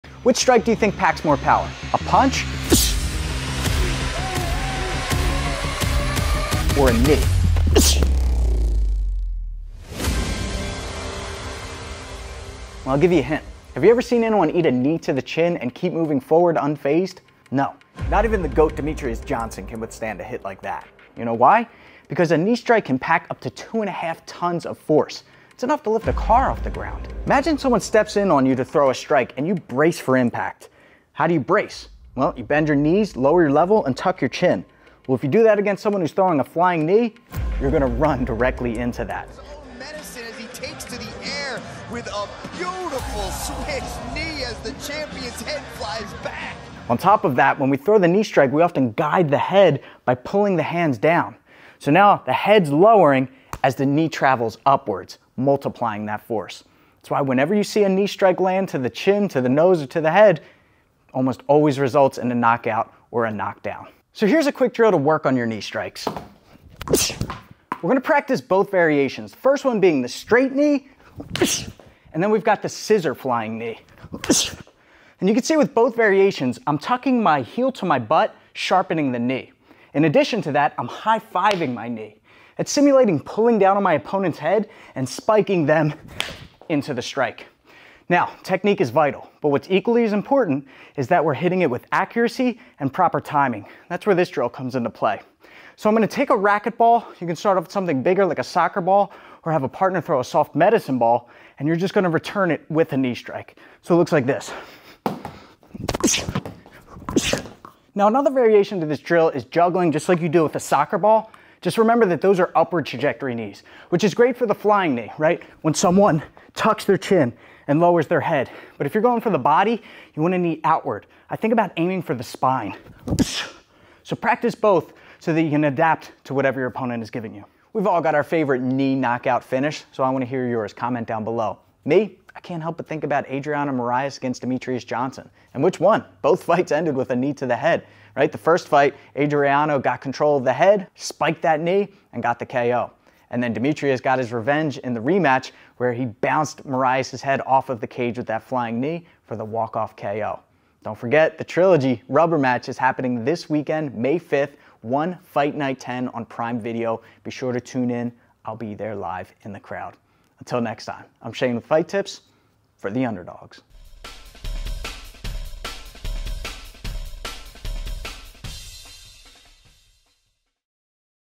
Flying Knees can Knock ANYONE Out
Nothing packs more force than a flying knee to the face -- like a magic button, this move will turn anyone off. In preparation for the Demetrious Johnson vs Adriano Moraes trilogy fight, we cover proper mechanics and drills to master your knee strike technique, timing, and accuracy for MMA.

I'm really curious who you think win this Friday at ONE FIGHT NIGHT 10 --

🤔DJ or Moraes⁉️

Tune in and look for me in the crowd at  on Prime Video, May 5 at 8pm ET.

ONEFightNight10 | Friday at 8PM ET/5PM PT